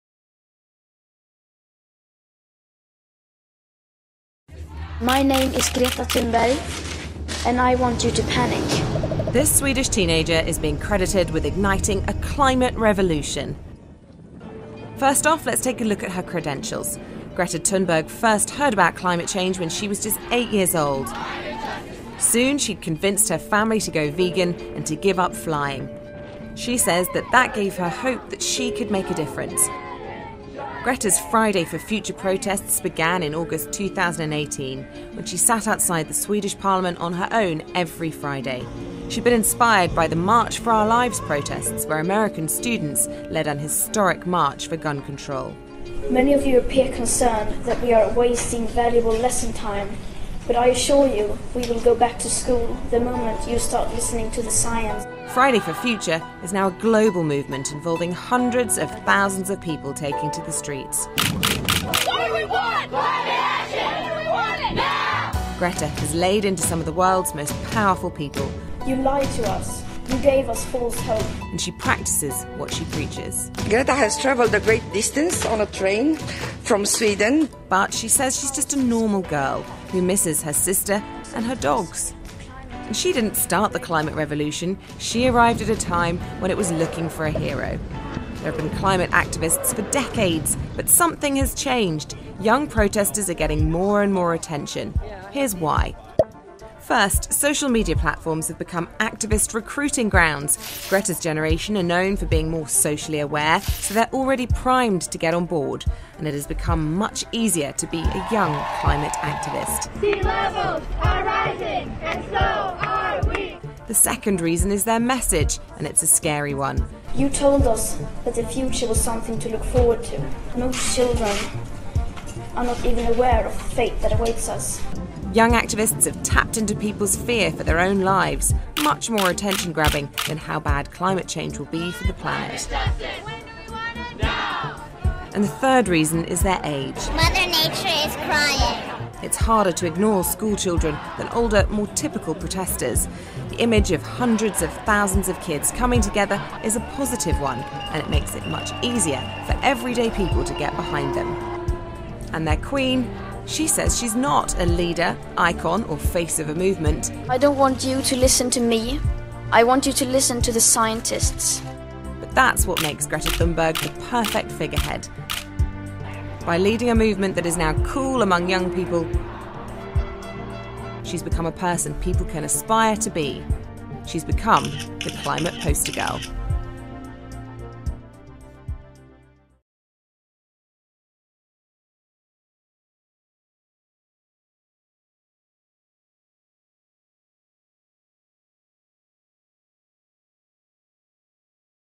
Greta Thunberg Chalk Art by Paul Arnold 2019
Sarasota Chalk Festival , Venice Florida Nov 15-17 , 2019  Greta Thunberg by Paul Arnold Fine Arts Graduate of Ringling College of Art and Design , The All Around Artist.